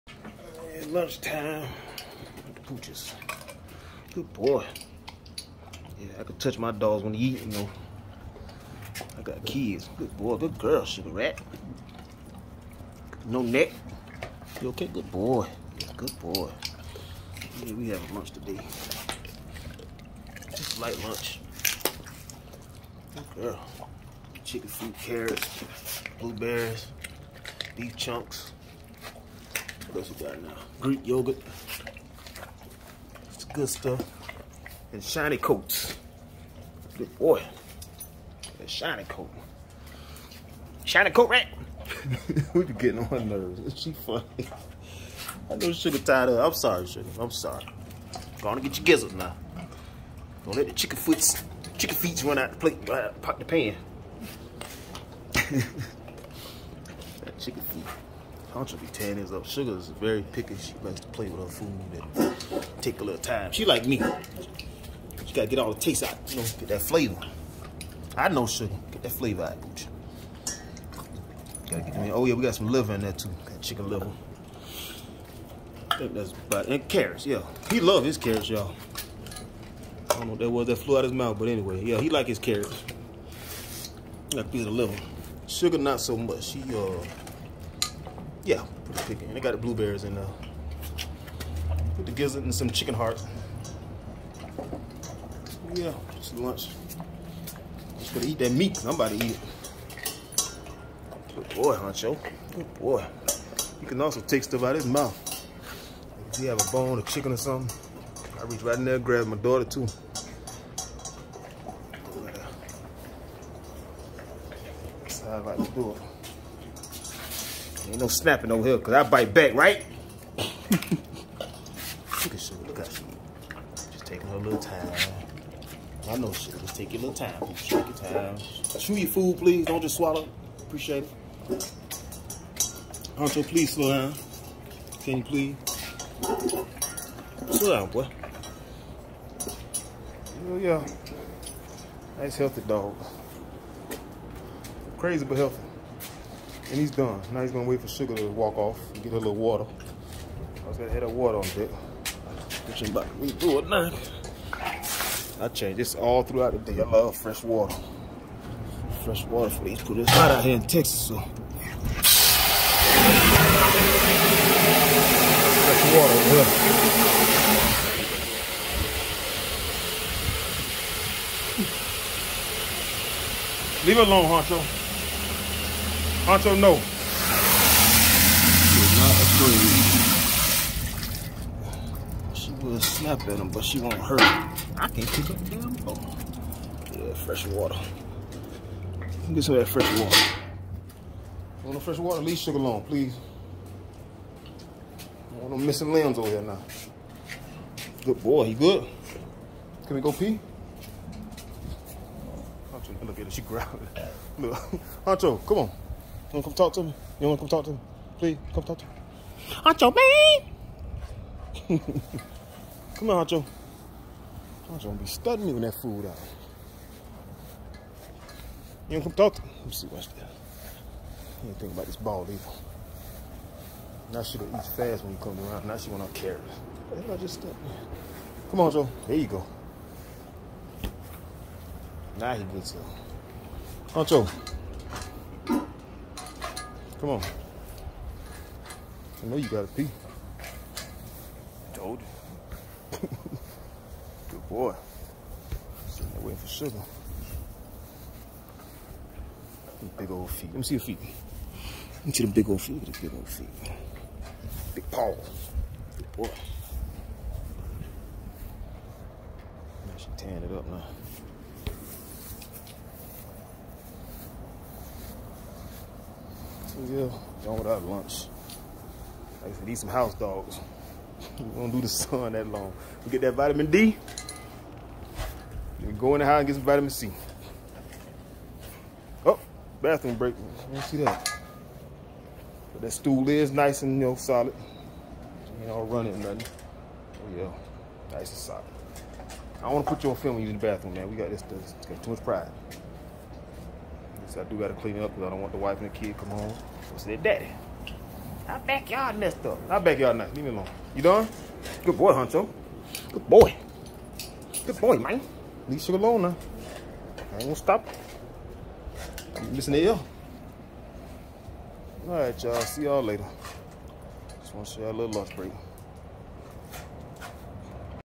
Raw Feeding Huncho (Boerboel) and Shuga (Bully Breed)
Hunch and Shuga having a nice raw lunch.